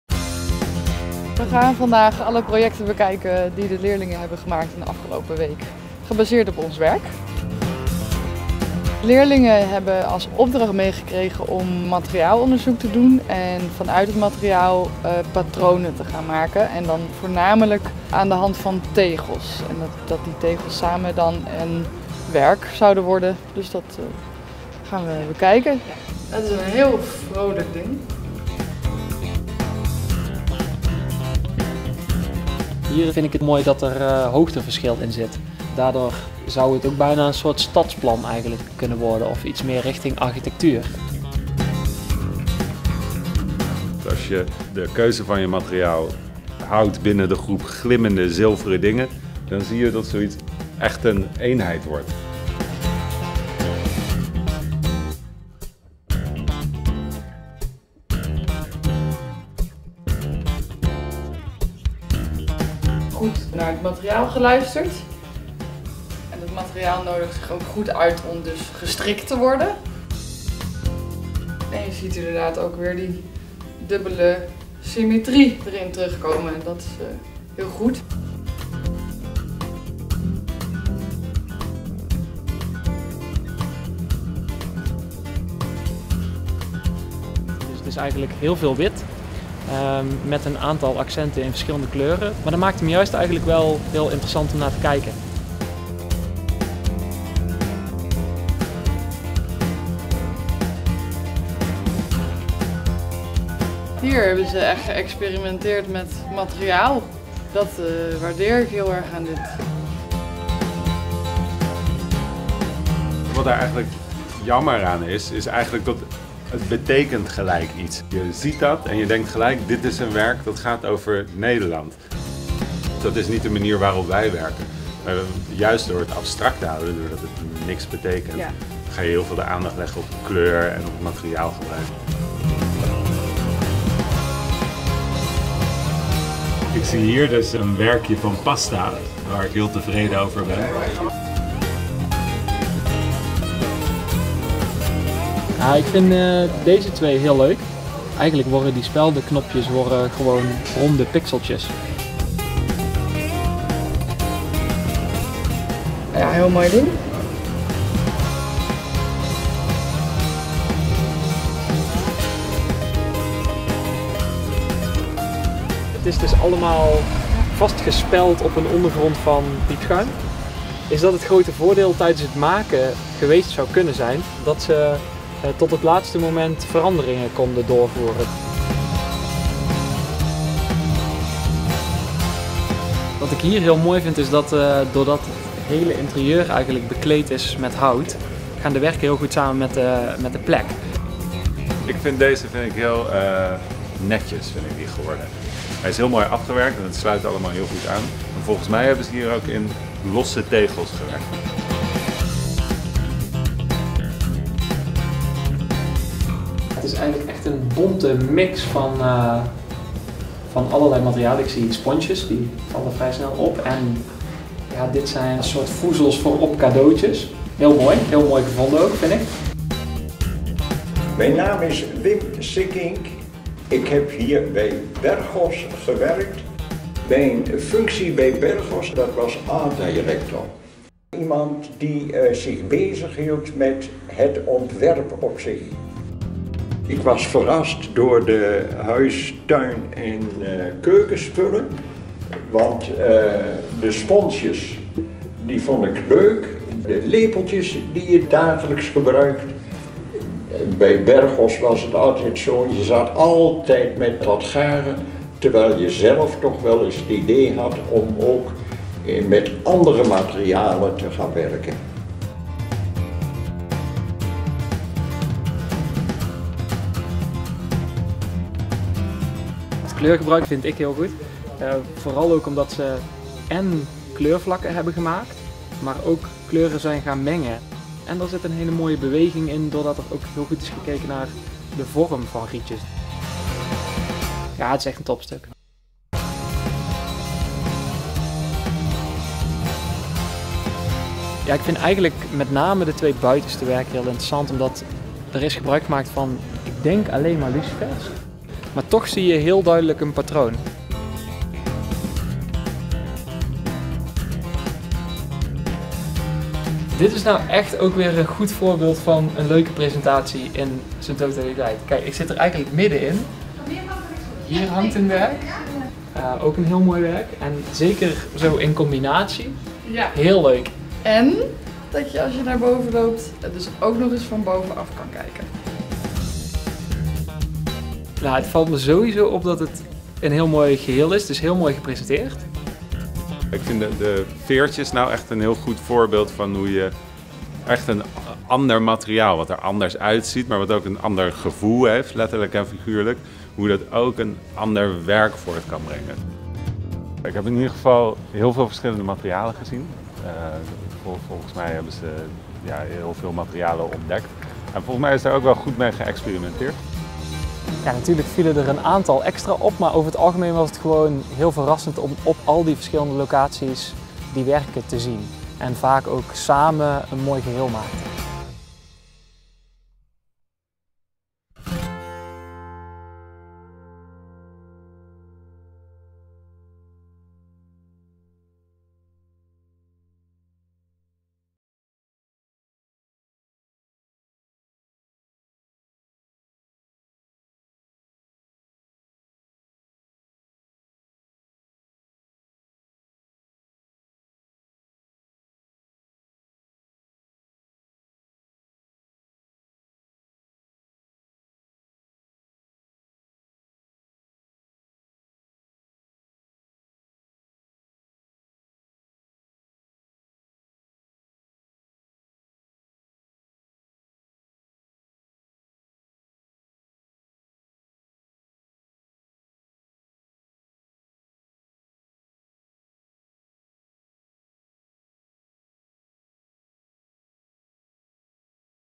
We Make Carpets bekijkt kunst-tapijten gemaakt door vmbo leerlingen uit Oss
Het kunsteducatie-project ‘Vmbo leerlingen maken kunst-tapijten’ vloeit voort uit de tentoonstelling van We Make Carpets ‘Something old, something new, something borrowed, something blue’, waarin ook ontwerp- en patroontekeningen van Bergoss te zien waren. Deze tentoonstelling was van 20 februari t/m 26 juni 2016 te zien in Museum Jan Cunen. ‘Jan Cunen in de stad – 2016’ was te zien van 1 april t/m 29 mei 2016. Het kunsteducatie-project en ‘Jan Cunen in de stad – 2016’ werd mogelijk gemaakt door de Gemeente Oss, Prins Bernhard Cultuurfonds, Fonds voor Cultuurparticipatie, middelbare school Het Hooghuis en Museum Jan Cunen.